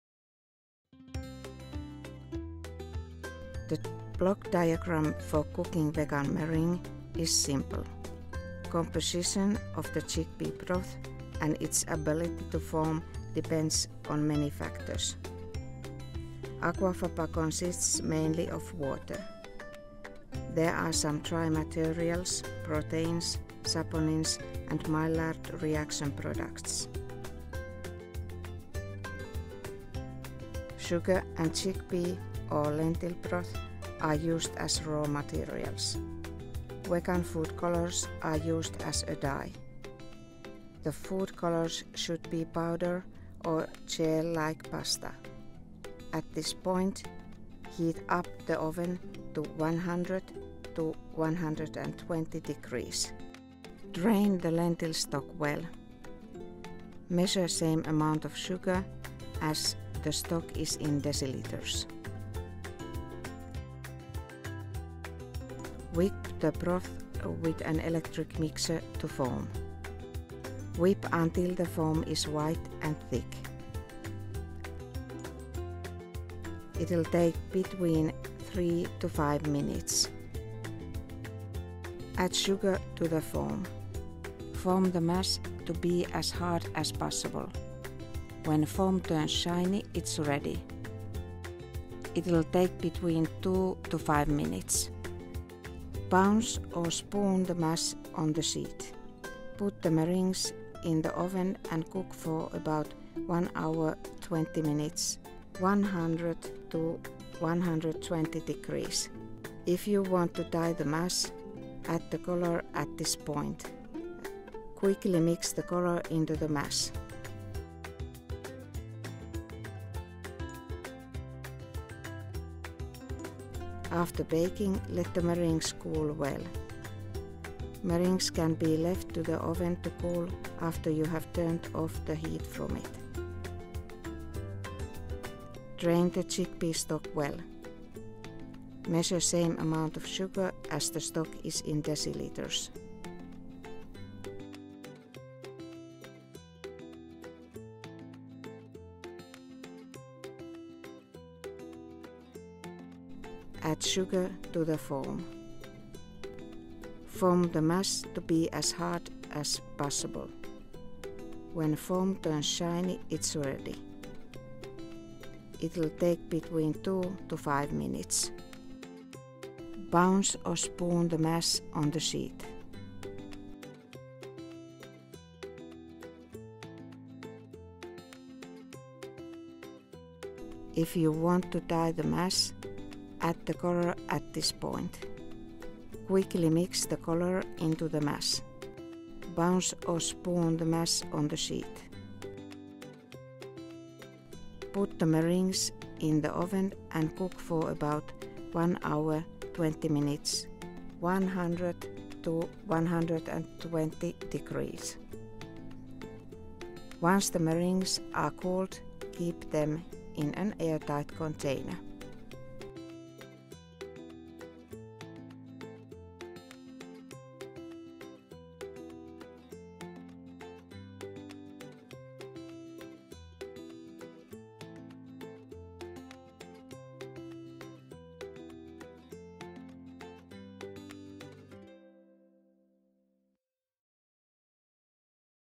Vegan Meringue | EQVEGAN
The demand for vegan products is increasing, which is why students and experts at Seinäjoki University of Applied Sciences (SeAMK) decided to produce a series of educational videos on how to prepare vegan products. The reasons why consumers use vegan products can be, for example, health or climate reasons. Health reasons are usually personal and can be due to an egg allergy or lactose intolerance. Reducing climate impact can be realised by choosing plant-based products, for example ecological and green choices. Regardless of whether the reason is the above or a combination of other reasons, we want to show you how to make meringues with whipped chickpea broth, i.e. aquafaba.
This video has been prepared by SeAMK, Seinäjoki University of Applied Sciences, within the EQVEGAN project ERASMUS+ Sector Skills Alliance project (621581-EPP-1-2020-1-PT-EPPKA2-SSA-EQVEGAN).